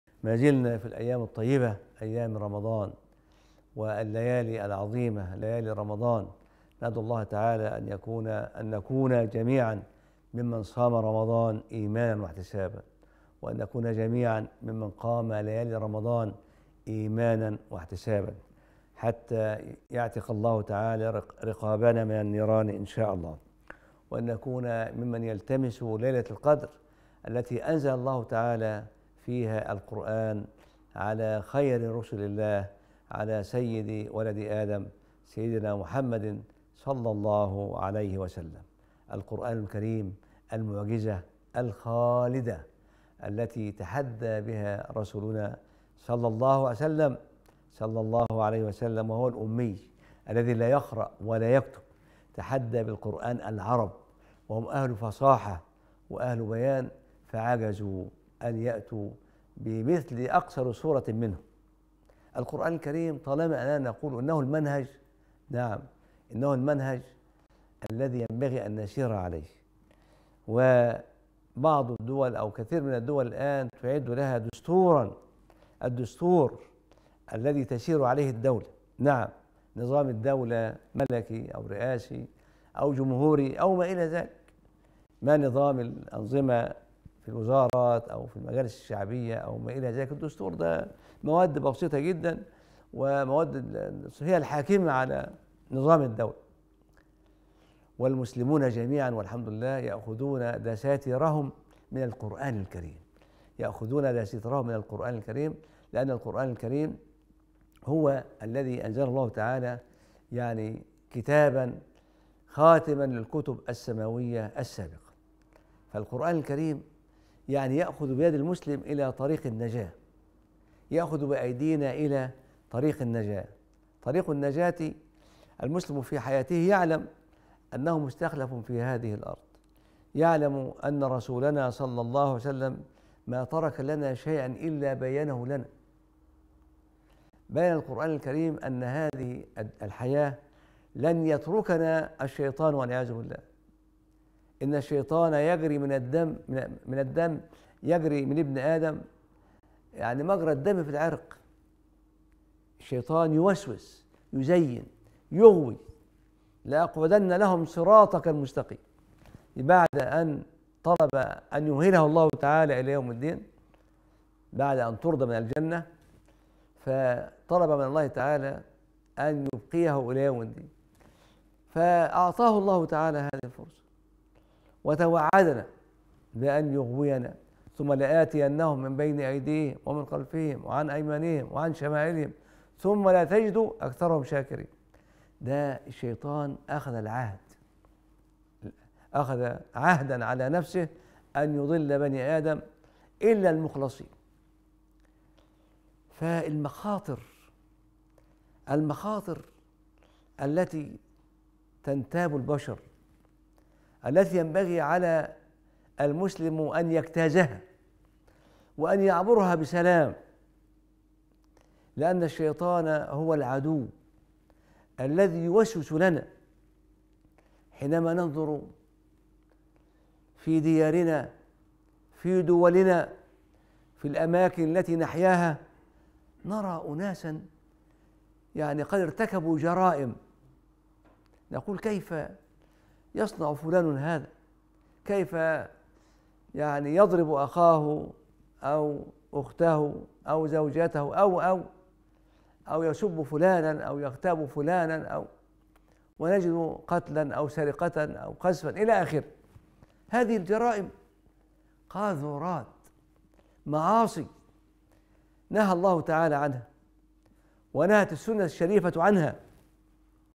تسامح ورحمة| "القرآن هو الدستور”.. عضو المنظمة العالمية لخريجي الأزهر يتحدث عن فضل العمل بالقرآن
تسامح ورحمة| "القرآن هو الدستور”.. عضو المنظمة العالمية لخريجي الأزهر يتحدث عن فضل العمل بالقرآن الكريم